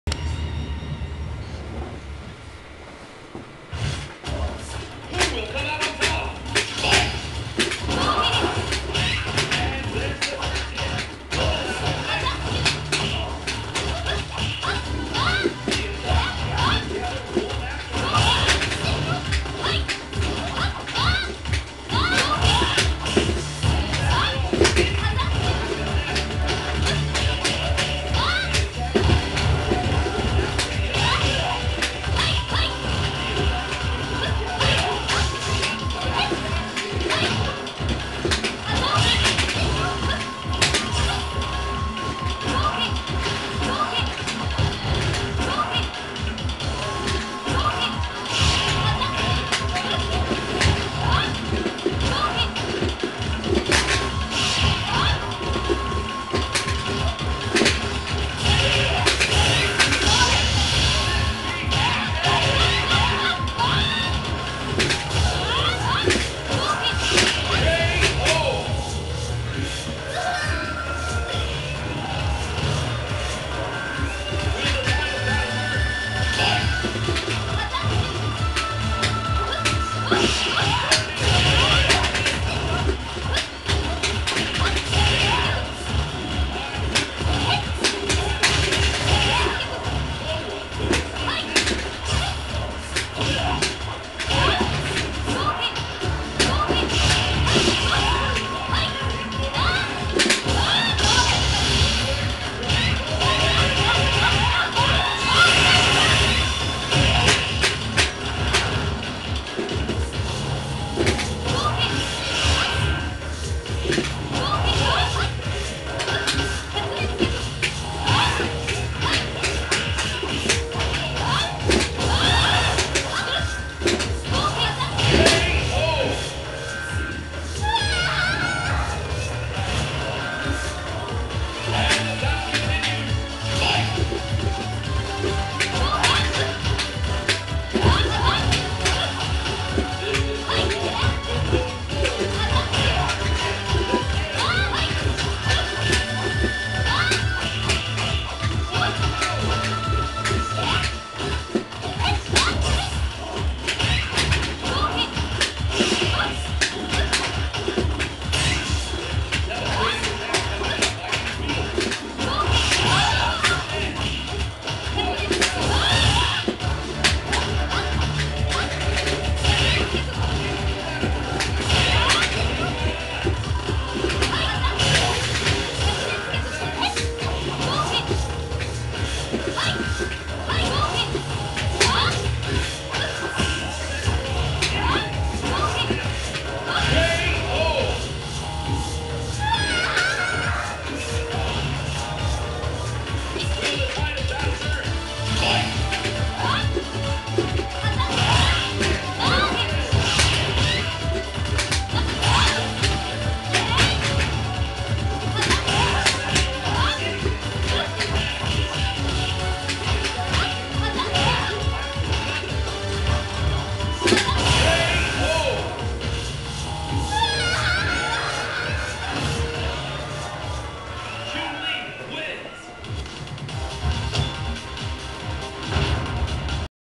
Skatan (CH) v Cpt Drannoc (CH) 8
A long set of online casuals between me and Cpt Drannoc.
Cpt Drannoc is learning Chun Li but plays Ken as his main.